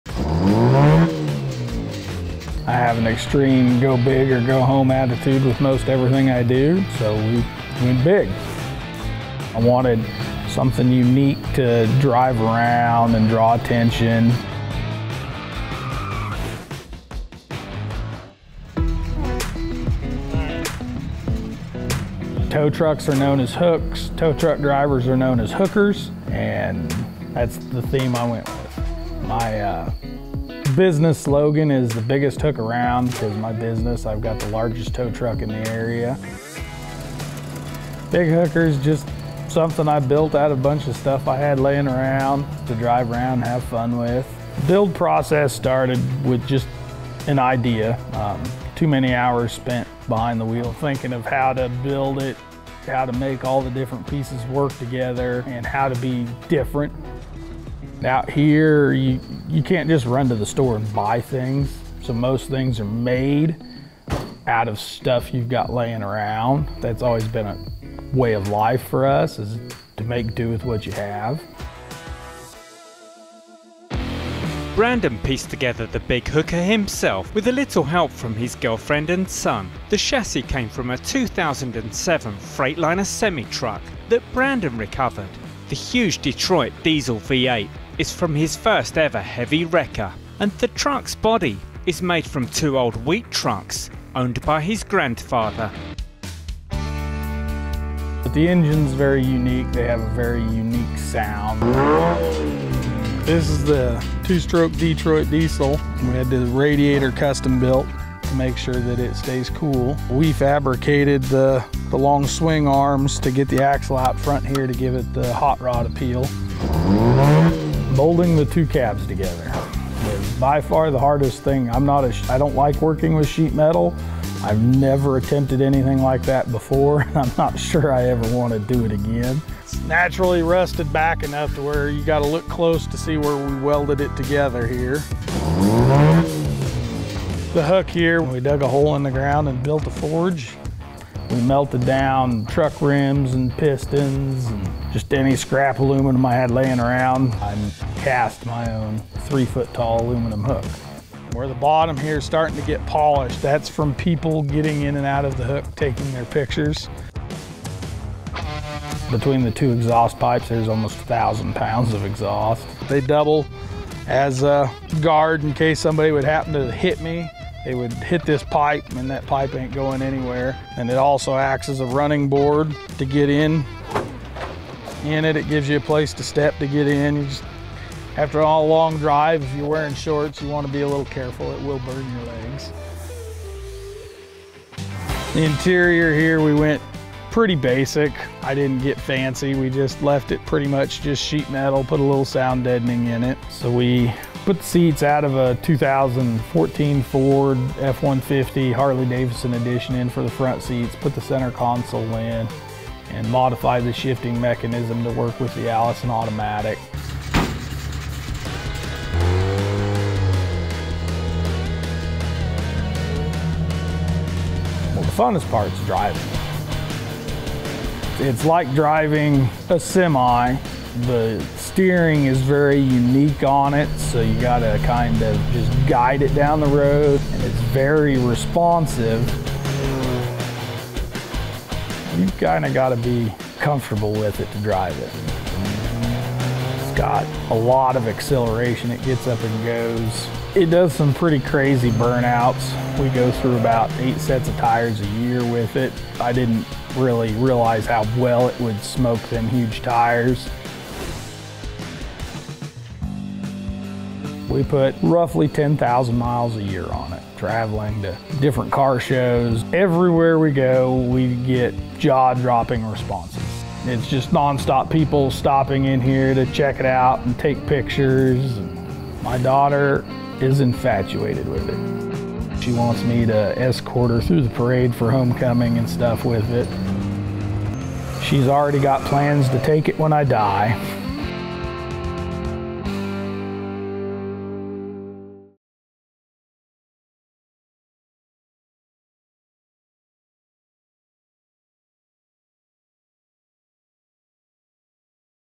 Big Hooker - The World's Most Insane Tow Truck | RIDICULOUS RIDES
A MAN from Kansas with a 'go big or go home' attitude has built one of the largest custom tow trucks in his area. Brandon put together the 'Big Hooker' using parts of old vehicles he had lying around. He spent many hours considering how to bring his idea to fruition, making all the different mismatched pieces work together. With the chassis of a 2007 Freightliner semi truck, a huge Detroit Diesel V8 engine came from Brandon's first ever heavy wrecker. Incredibly, the body is actually made from two separate old wheat trucks that were owned by his grandfather. Brandon told Ridiculous Rides: "Moulding the two cabs together is by far the hardest thing, I've never attempted anything like that before, I'm not sure I ever want to do it again."

Producer : Giacomo Brunelli, Ruby Coote
Editor : Will Strange